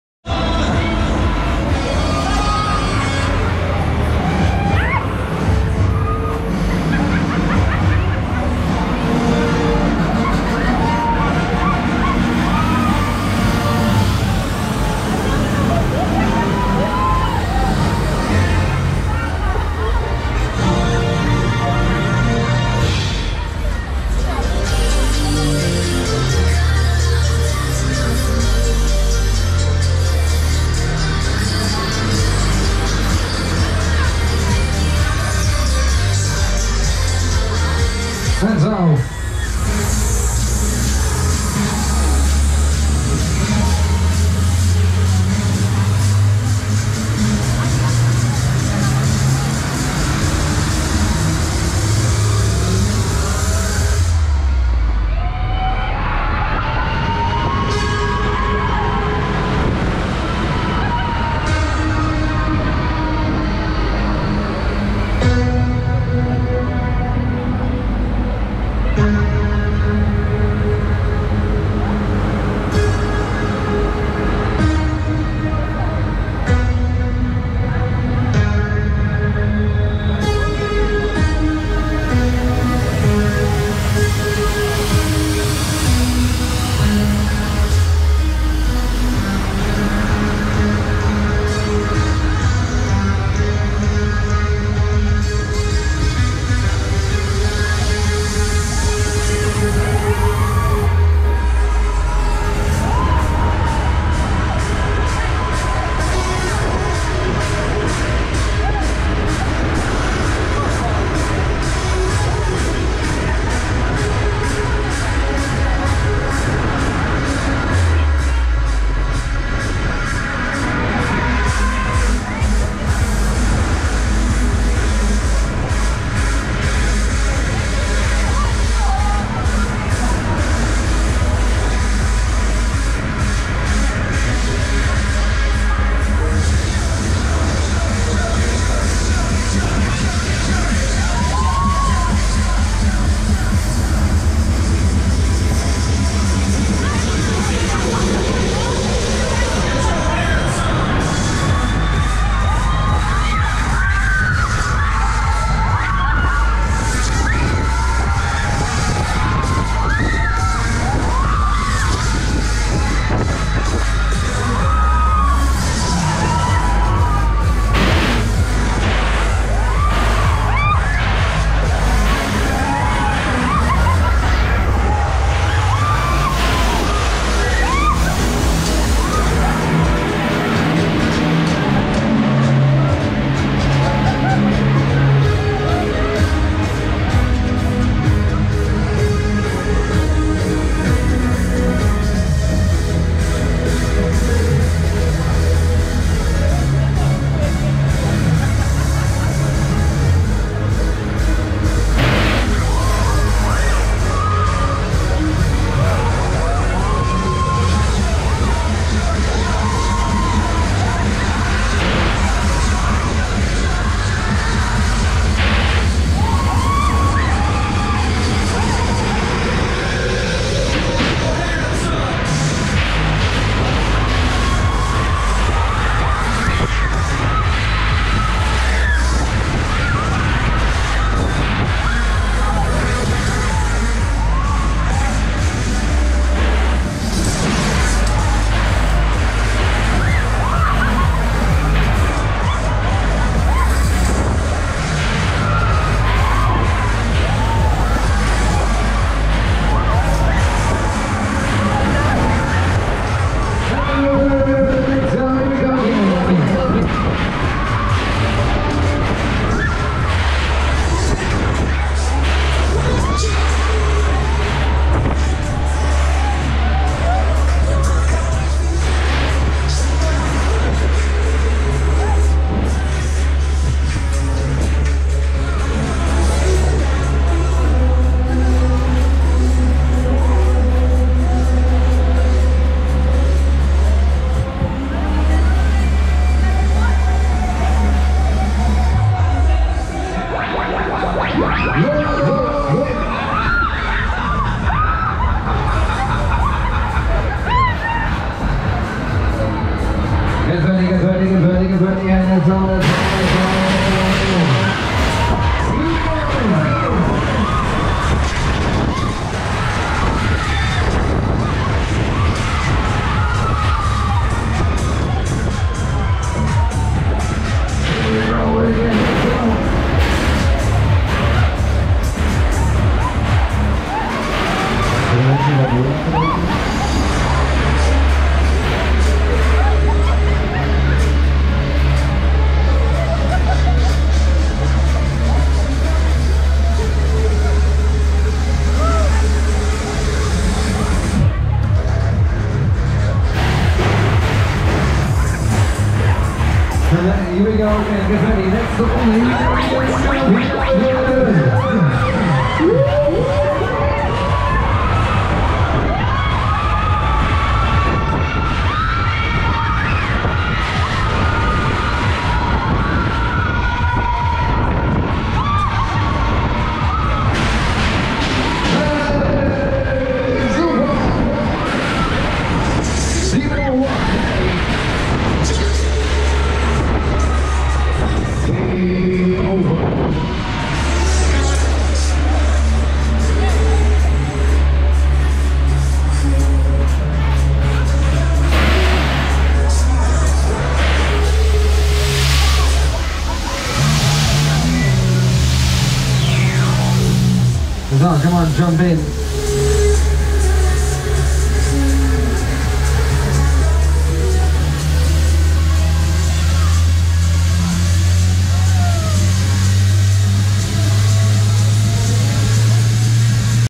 Karuzela Dance Jumper (Ruitenberg-Sipkema) On ride 4 - Szczecin - Holiday Park 2023
Zobaczcie czwarty film z Dance Jumpera w Holiday Parku! :)